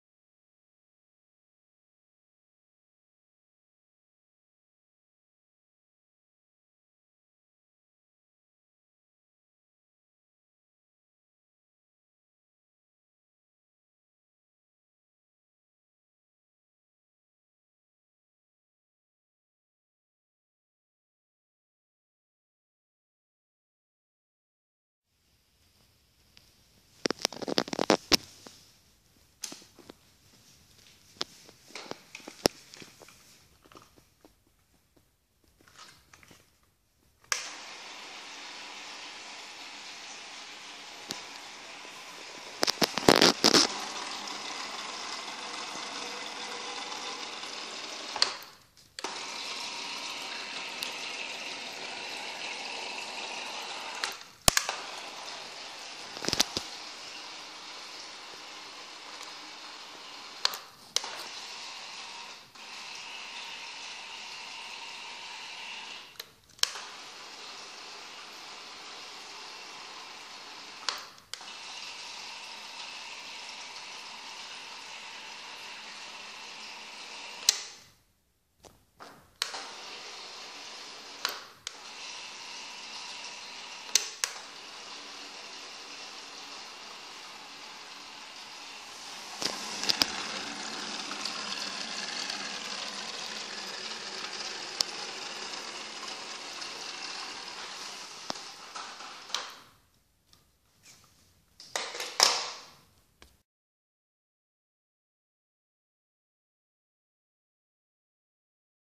Remote Controlled Raft
Remote Controlled Raft made from water bottles, a remote controlled car, and Legos. The raft can move both forwards and backwards.

This clip was made with Windows Movie Maker for Windows 8.1